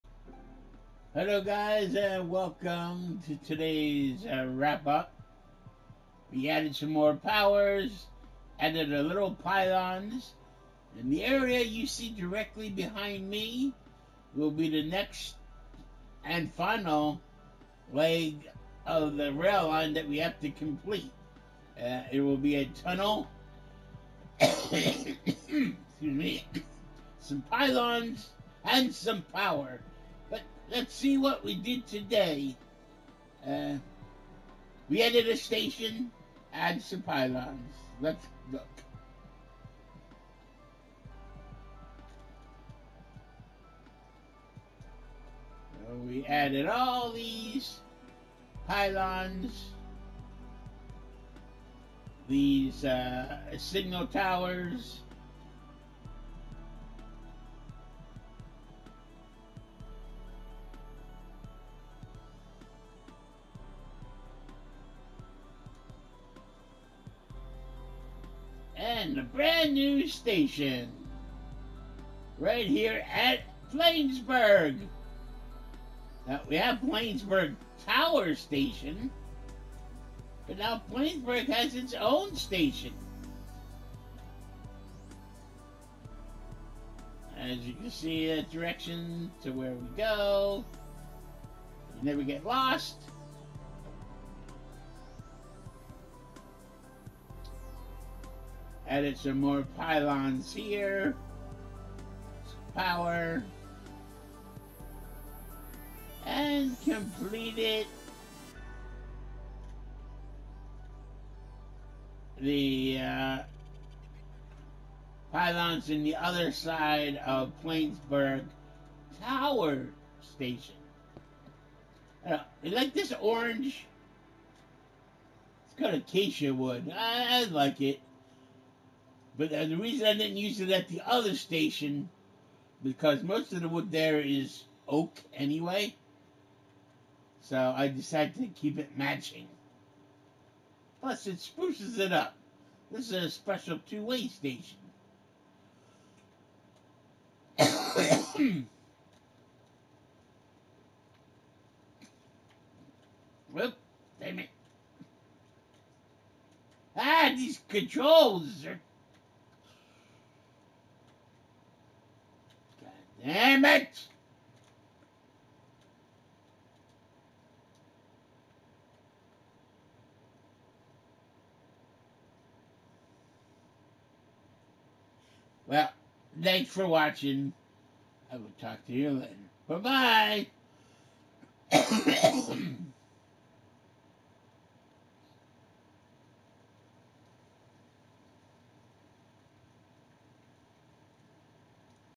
New Second Plainsburg Station Added, Power and Pylons
Today I added the second Plainsburg Station more signal towers and power pylons.